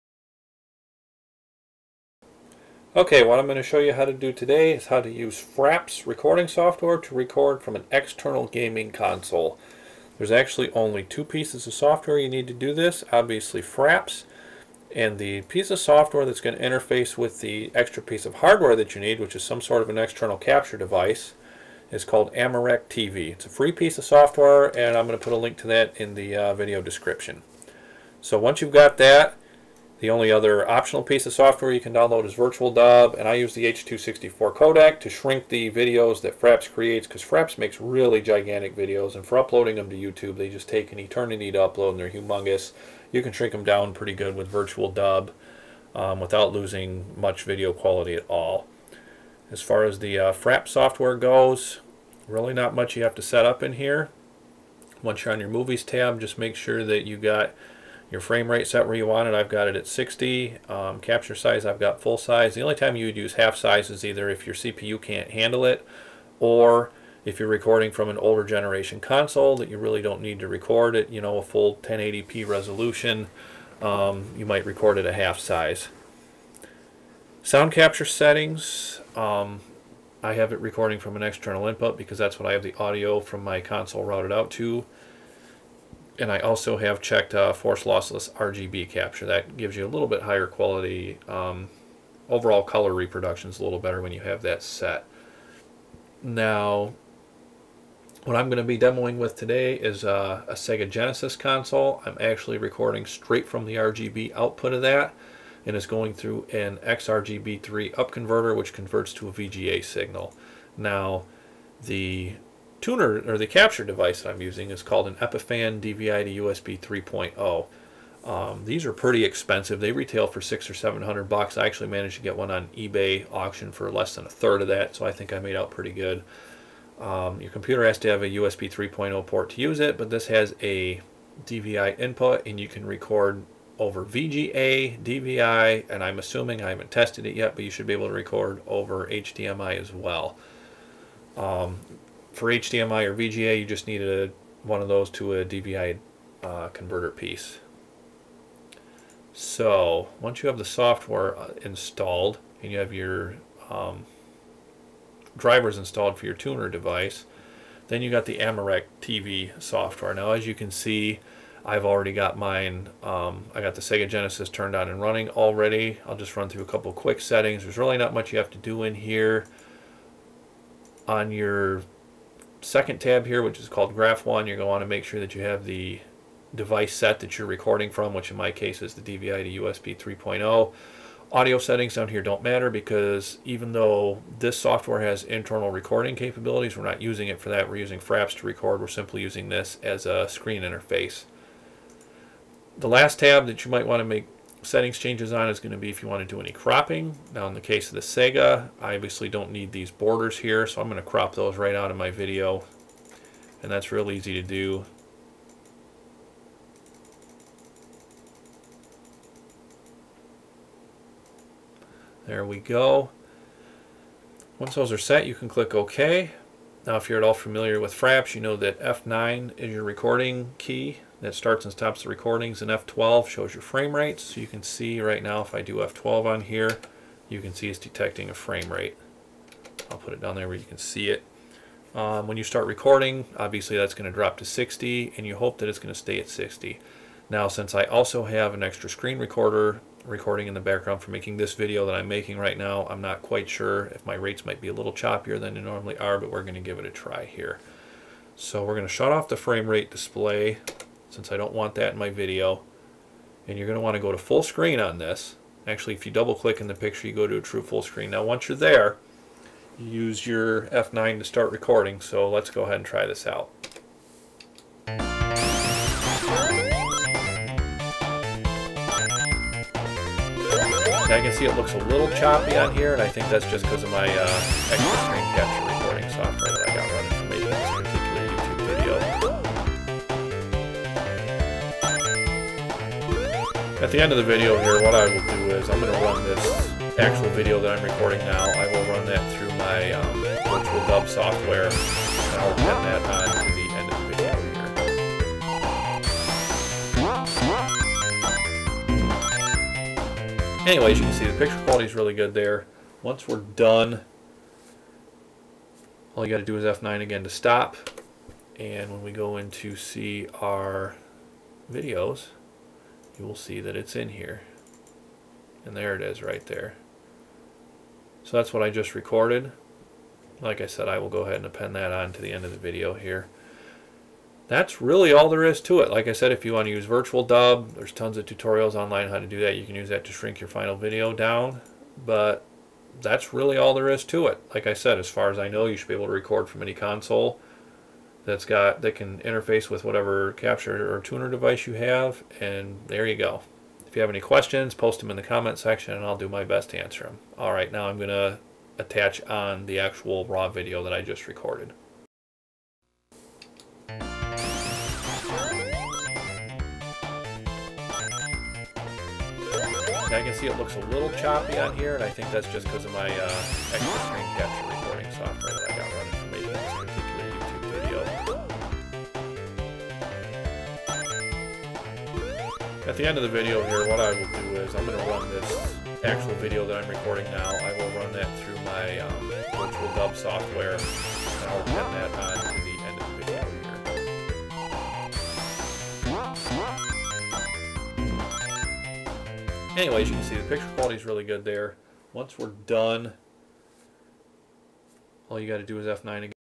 How to use FRAPS to Record / Capture External Video Game Consoles
Here is an explanation on how to record any external video source with FRAPS (software), AmaRecTV (software), and any tuner/capture card (in my case, Epiphan DVI2USB)